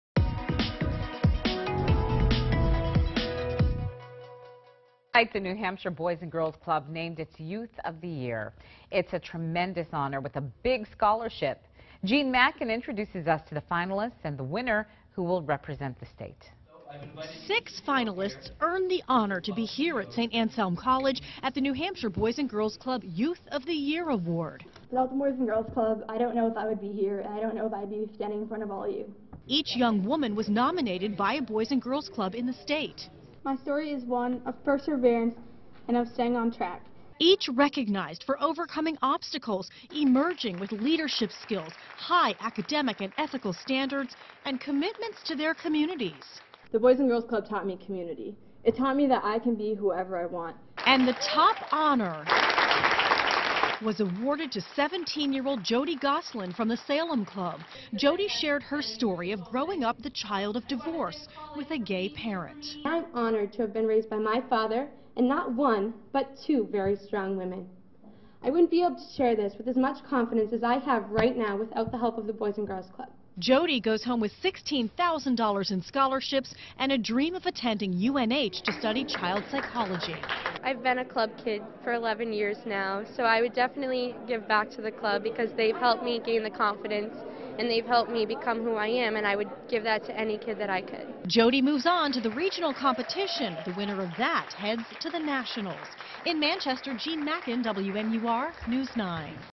Boys & Girls Club names Youth of the Year
The New Hampshire Boys & Girls Club named its Youth of the Year on Thursday. WMUR's Jean Mackin reports.